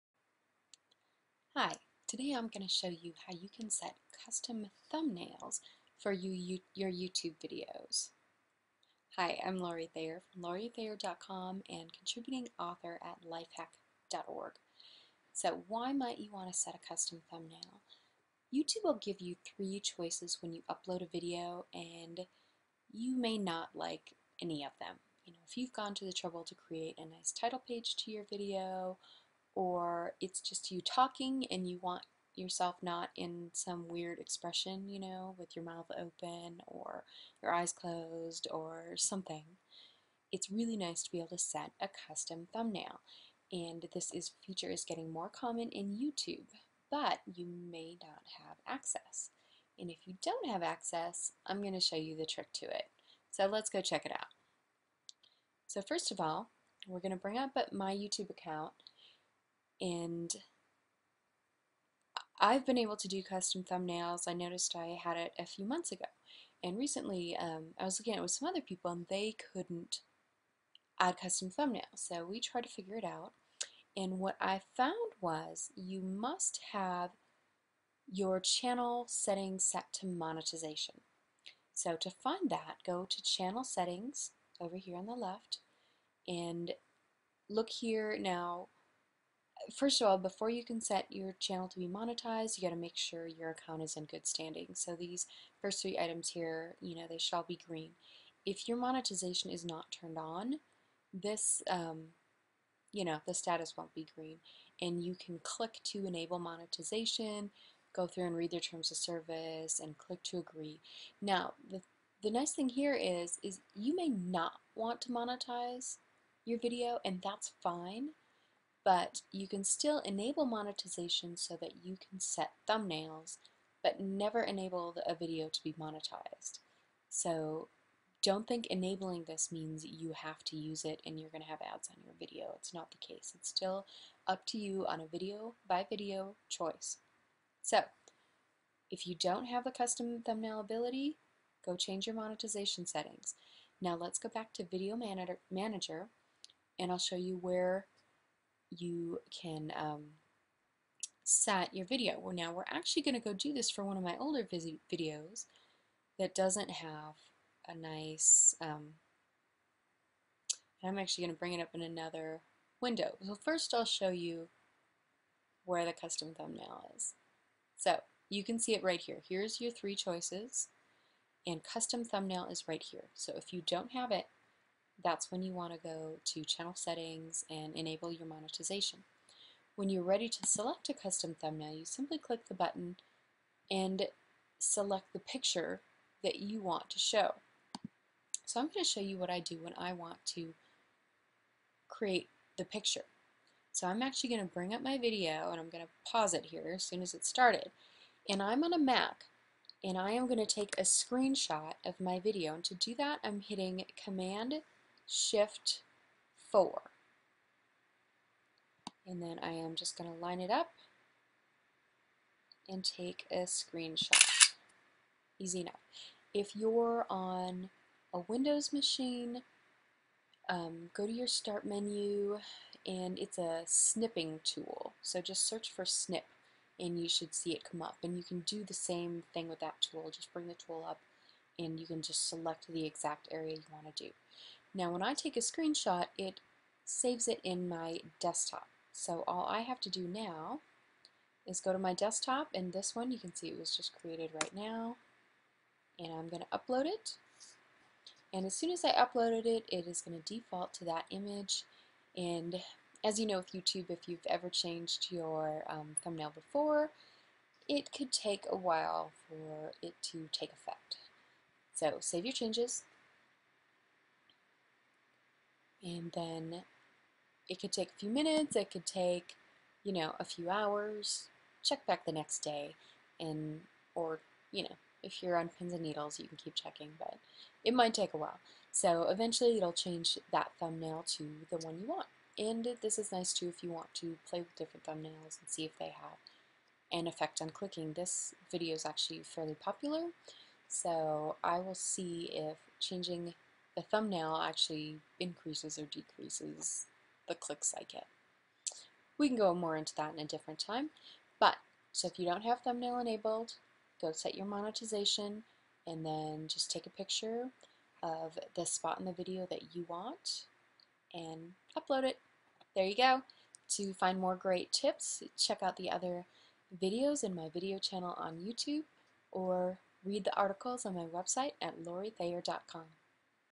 How To Set A Custom Thumbnail In YouTube
Wondering how you set your video's thumbnail to a picture you want? It's possible and even if you don't have the ability right now, I'll show you how to enable it in this tutorial.